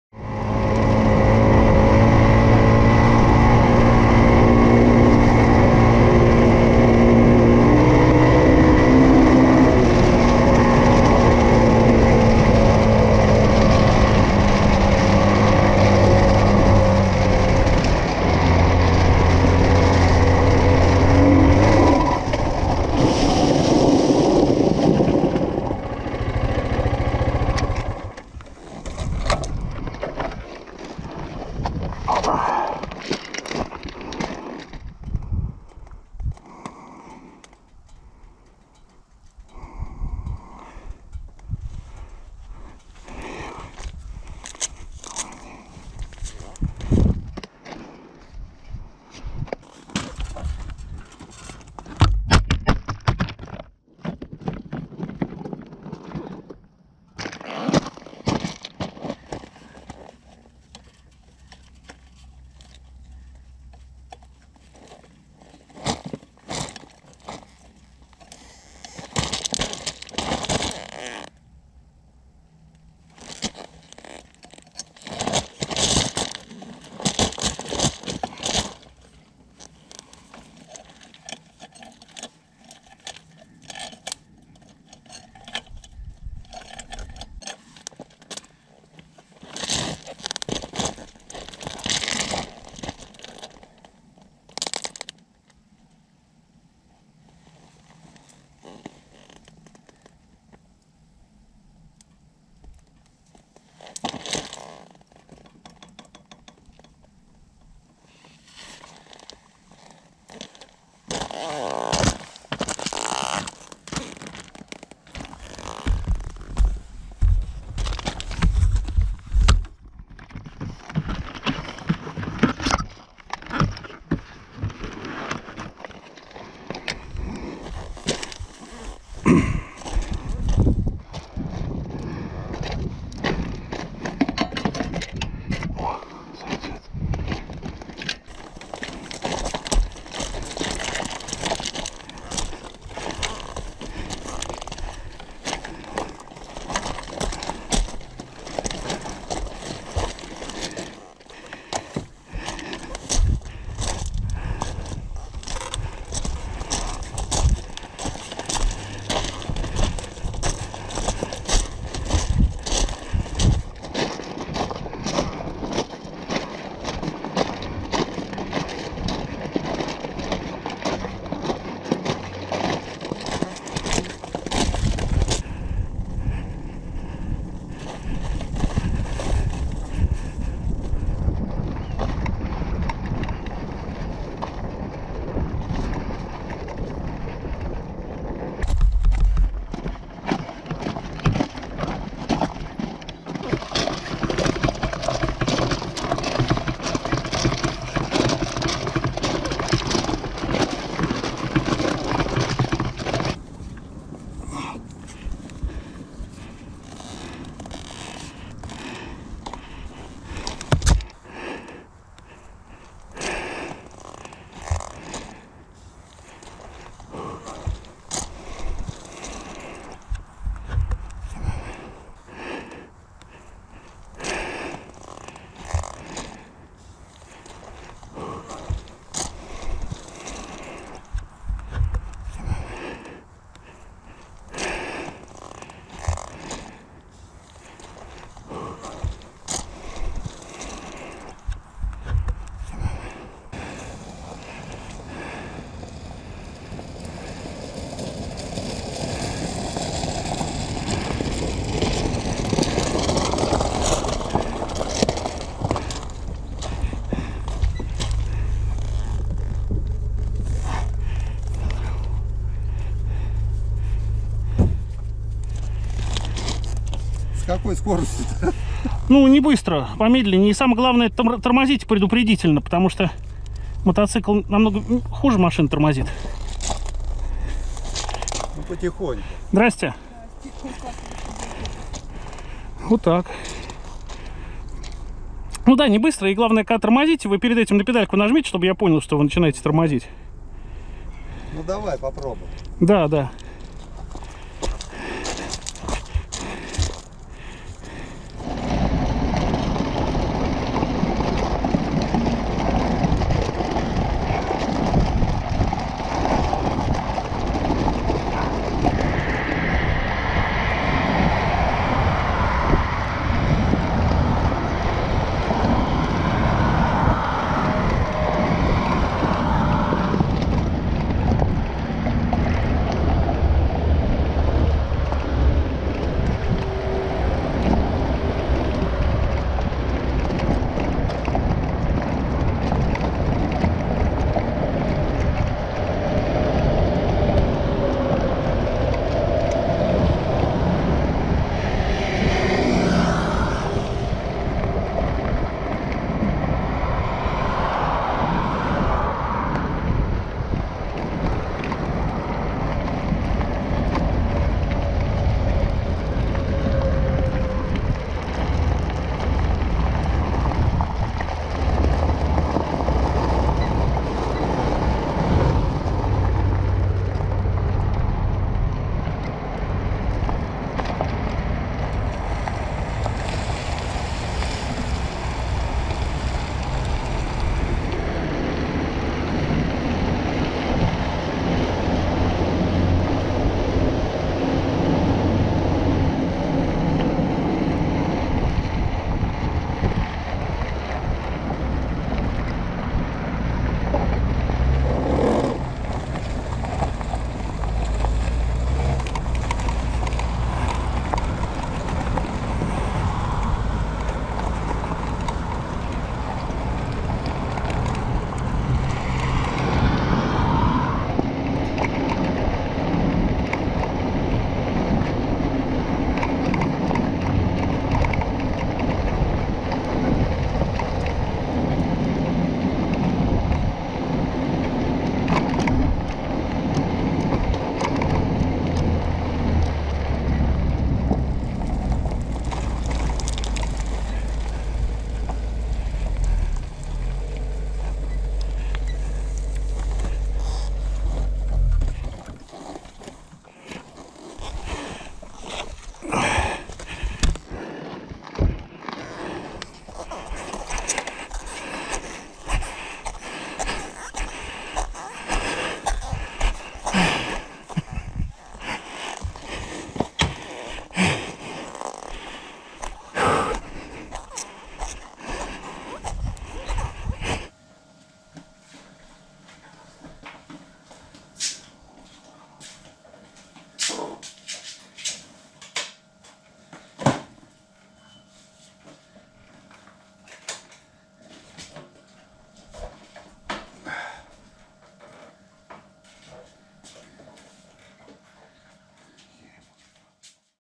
Разрыв цепи на гравии с блокировкой колеса | Буксировка мотоцикла автомобилем
Момент разрыва 0:18
Motoland XR250 ENDURO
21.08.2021